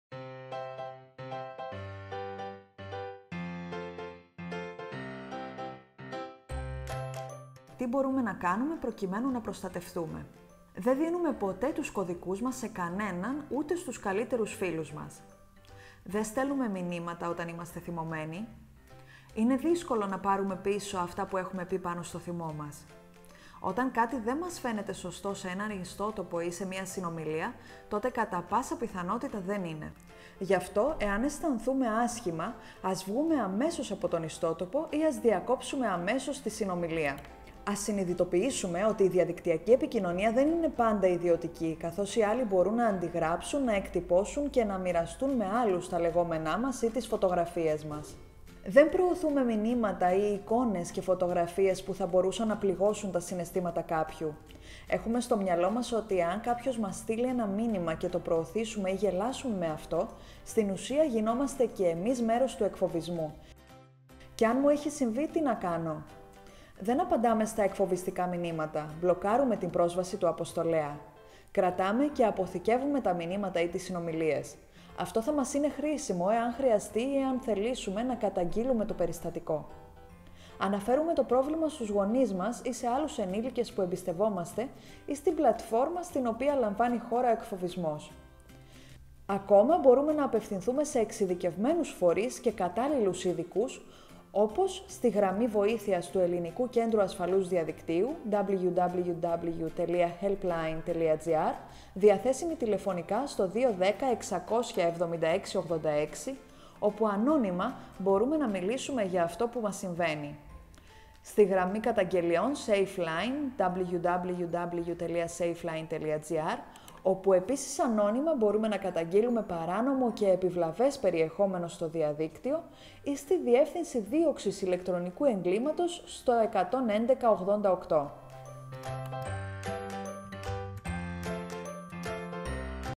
Τι μπορούμε να κάνουμε προκειμένου να προστατευτούμε;
Δε δίνουμε ποτέ τους κωδικούς μας σε κανέναν, ούτε στους καλύτερους φίλους μας.

Δεν στέλνουμε μηνύματα όταν είμαστε θυμωμένοι. Είναι δύσκολο να πάρουμε πίσω αυτά που έχουμε πει πάνω στο θυμό μας.

Όταν κάτι δε μας φαίνεται σωστό σε έναν ιστοχώρο ή σε μια συνομιλία, τότε κατά πάσα πιθανότητα δεν είναι. Γι’ αυτό, εάν αισθανθούμε άσχημα, ας βγούμε αμέσως από τον ιστοχώρο ή ας διακόψουμε αμέσως τη συνομιλία.

Ας συνειδητοποιήσουμε ότι η διαδικτυακή επικοινωνία δεν είναι πάντα ιδιωτική, καθώς οι άλλοι μπορούν να αντιγράψουν, να εκτυπώσουν και να μοιραστούν με άλλους τα λεγόμενά μας ή τι φωτογραφίες μας.

Δεν προωθούμε μηνύματα ή εικόνες και φωτογραφίες που θα μπορούσαν να πληγώσουν τα συναισθήματα κάποιου. Έχουμε στο μυαλό μας ότι, εάν κάποιος μας στείλει ένα μήνυμα και το προωθήσουμε ή γελάσουμε με αυτό, στην ουσία γινόμαστε και εμείς μέρος του εκφοβισμού

Κι αν μου έχει συμβεί τι να κάνω;

Δεν απαντάμε στα εκφοβιστικά μηνύματα. Μπλοκάρουμε την πρόσβαση του αποστολέα.
Κρατάμε και αποθηκεύουμε τα μηνύματα ή τις συνομιλίες. Αυτό θα μας είναι χρήσιμο εάν χρειαστεί ή εάν θελήσουμε να καταγγείλουμε το περιστατικό.
Αναφέρουμε το πρόβλημά μας στους γονείς μας ή σε άλλους ενήλικες που εμπιστευόμαστε ή στην πλατφόρμα στην οποία λαμβάνει χώρα ο εκφοβισμός.
Η μπορούμε να απευθυνθούμε σε εξειδικευμένους φορείς και κατάλληλους ειδικούς όπως

στη Γραμμή Βοηθείας Ελληνικού Κέντρου Ασφαλούς Διαδικτύου διαθέσιμη τηλεφωνικά στο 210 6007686 όπου ανώνυμα μπορώ να μιλήσω για αυτό που μου συμβαίνει
Στη Γραμμή καταγγελιών SafeLine όπου επίσης ανώνυμα μπορώ να καταγγείλω παράνομο και επιβλαβές περιεχόμενο στο διαδίκτυο
Ή στη Διεύθυνση Δίωξης Ηλεκτρονικού Εγκλήματος στο 11188